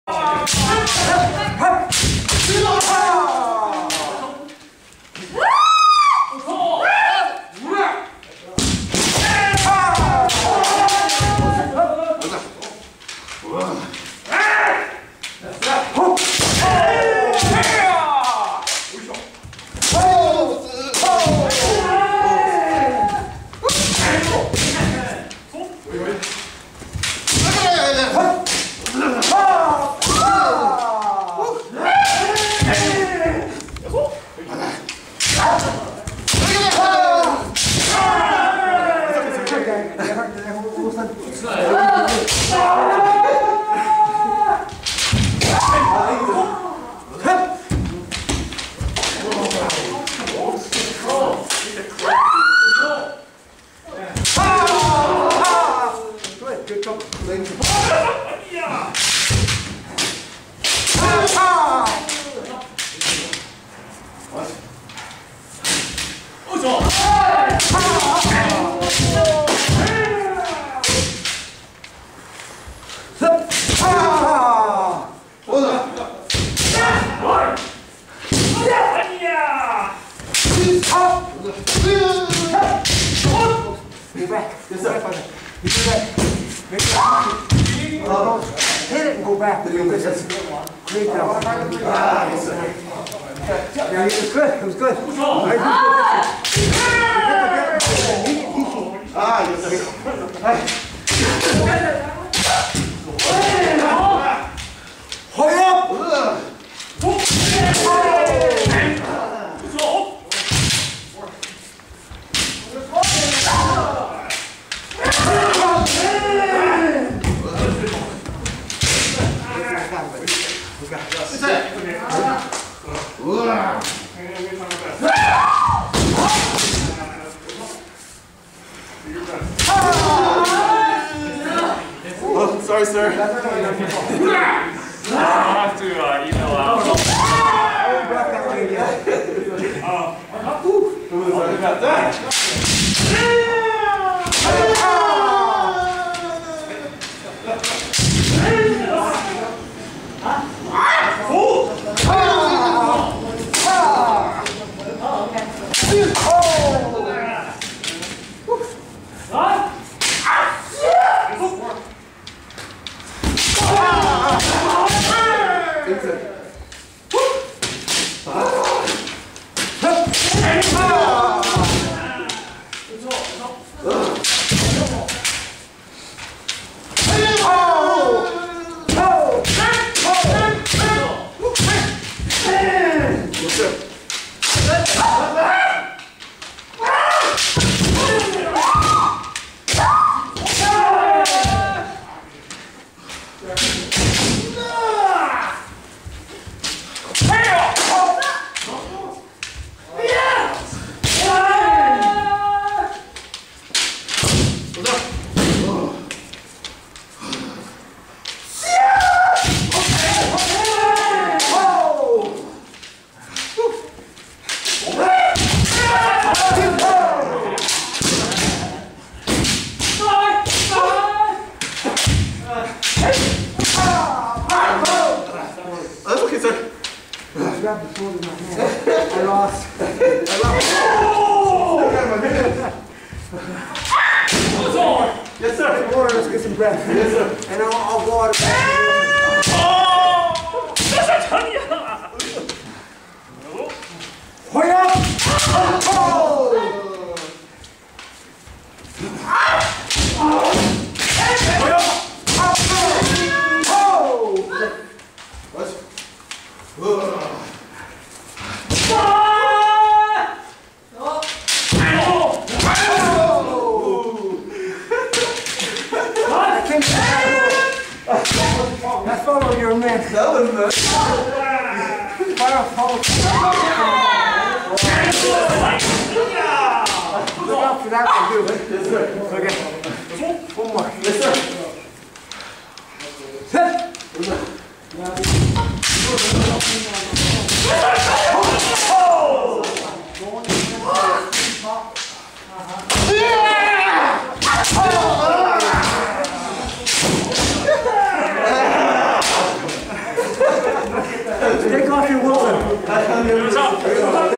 Kendo Training in San Diego: Jigeiko 4
Kendo Jigeiko in San Diego with Sensei. More destruction of my Do! Training in Jodan No Kamae against strong Head Sensei of San Diego! Exciting, nonstop action kendo dojo Jigeiko